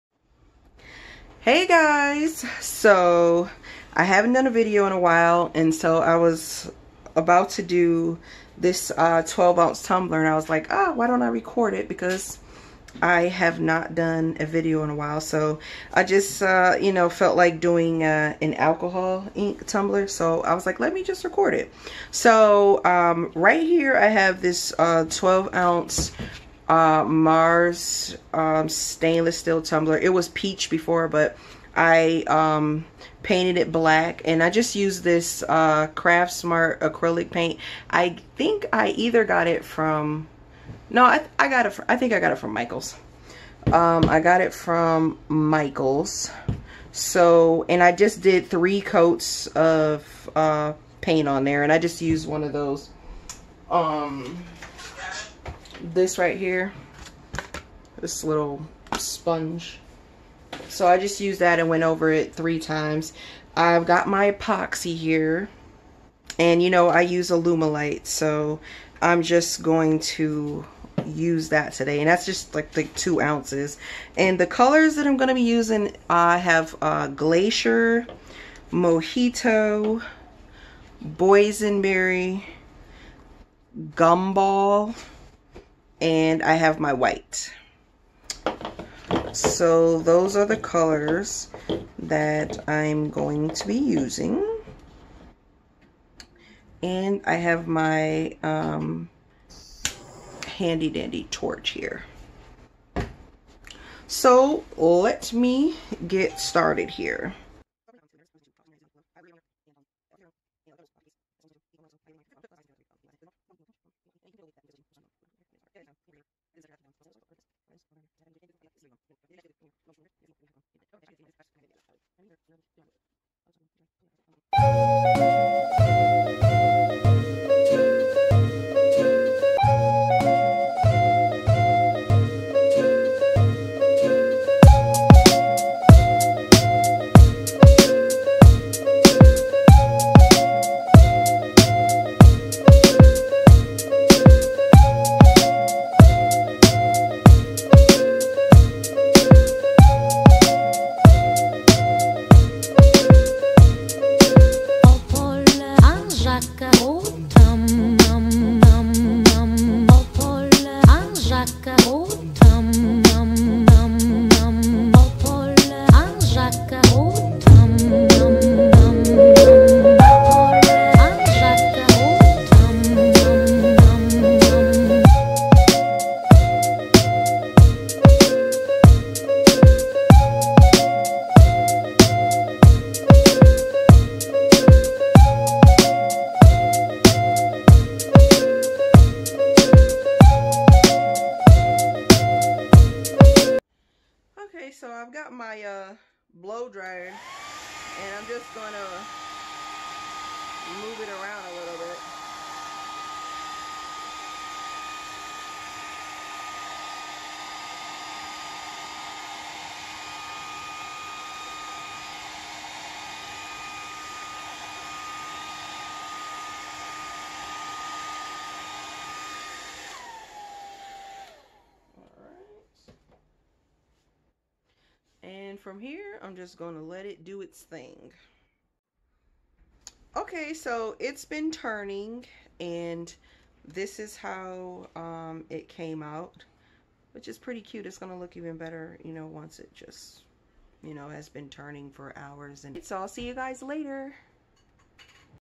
EASY ALCOHOL INK TUMBLER
Hello everybody. This is a quick project you can do. Alcohol ink tumblers are creative and unique.
You just need Epoxy, Acrylic Paint, Alcohol ink and a Tumbler of your choice. Please enjoy.
Be sure to leave any comments or questions!